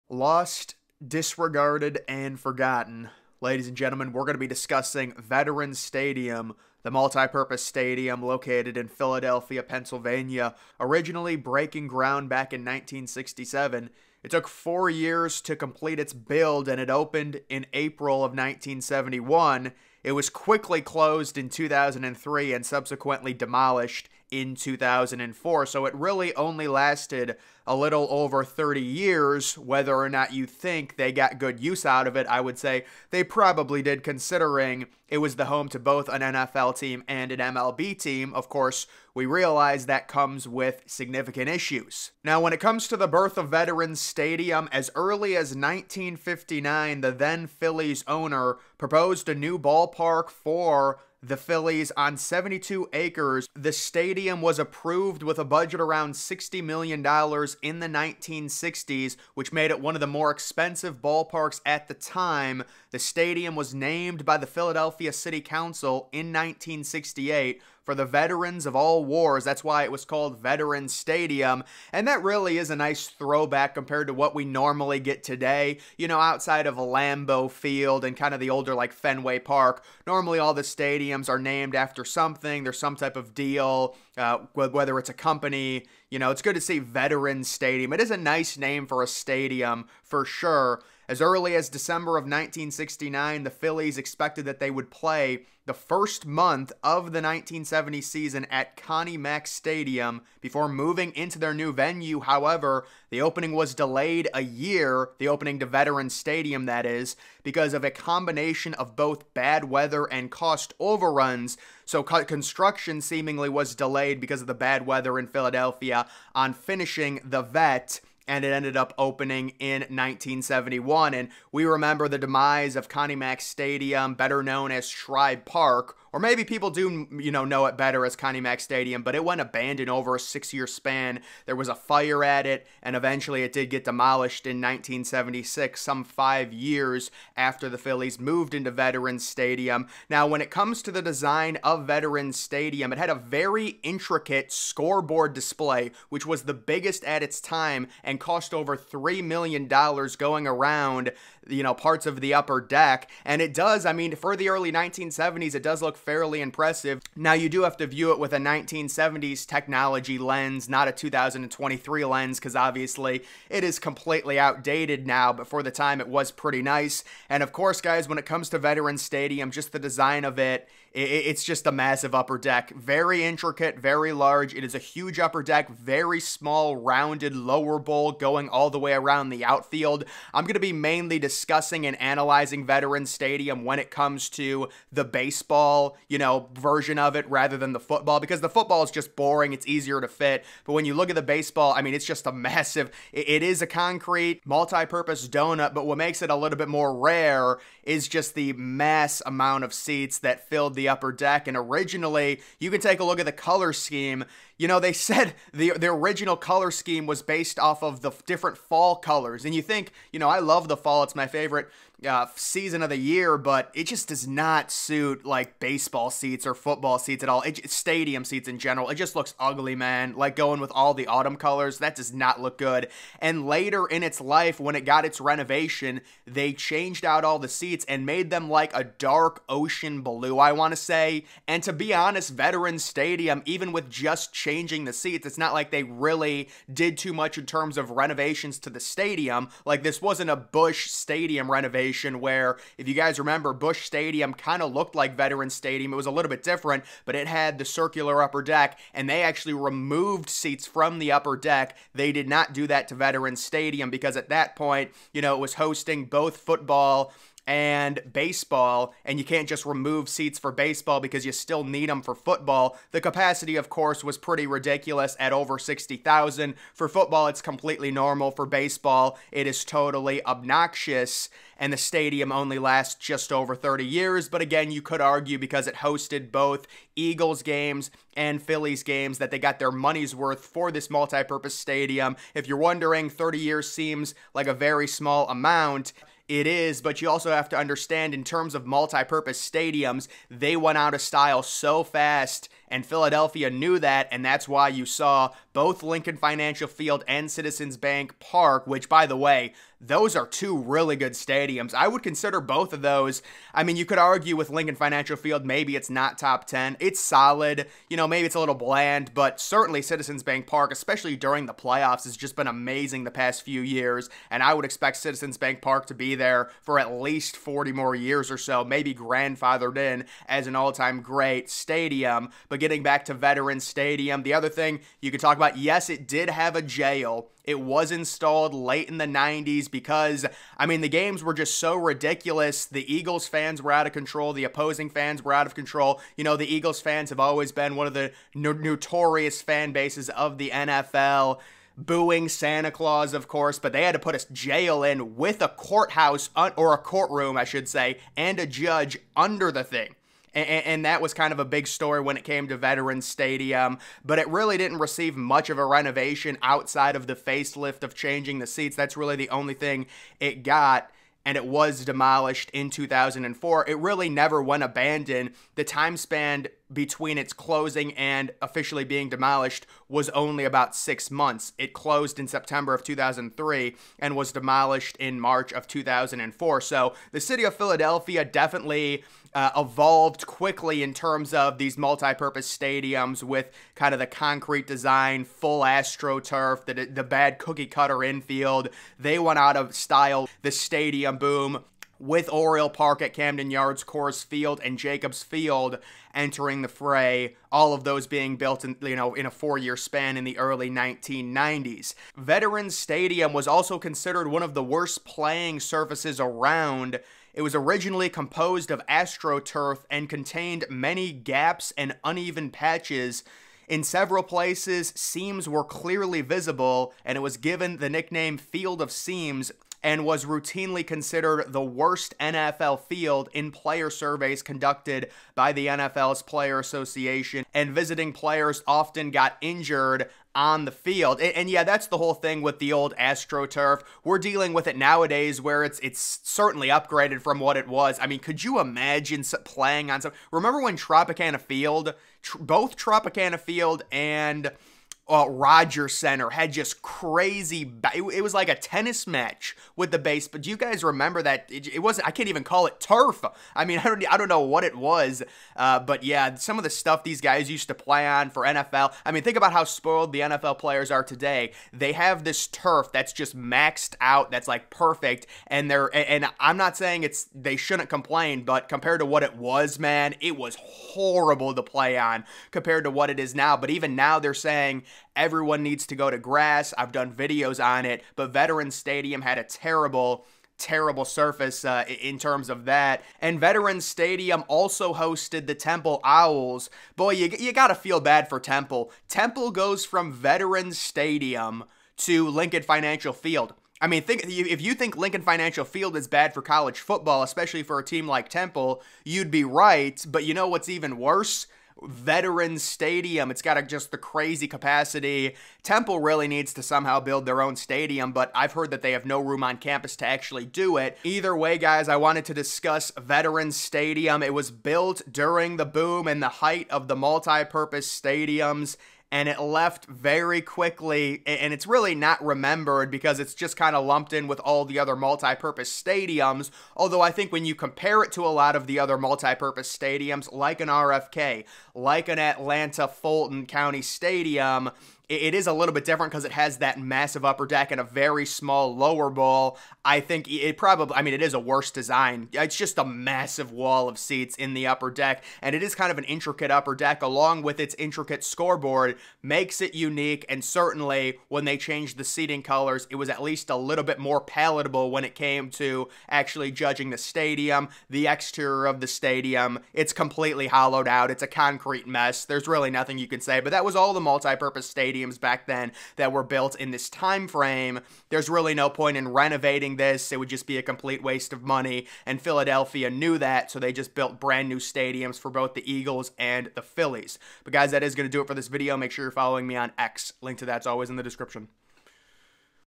Lost & Disregarded: Veterans Stadium
Discussing Veterans Stadium, home of the Philadelphia Phillies and Eagles